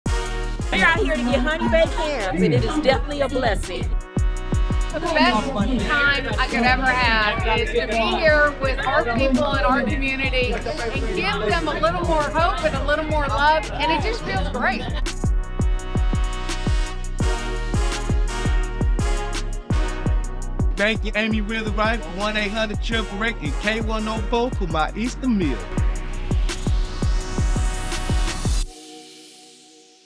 Dallas Ham Giveaway with Witherite Law Group
Discover the joy of giving with Witherite Law Group this Easter 🐣. We’re spreading cheer with 2,000 Easter feasts for families in need.